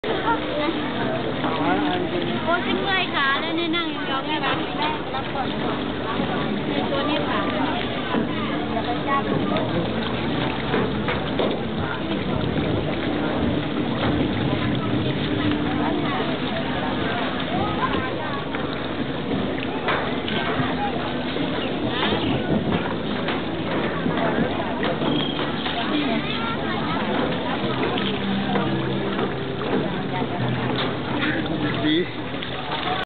Hand Feeding Catfish
There is a Thai Buddhist to feed fish at the rivers near their Temples. THis lady was hand feeding them bread. For Photos, Blogs and Thailand Travel information go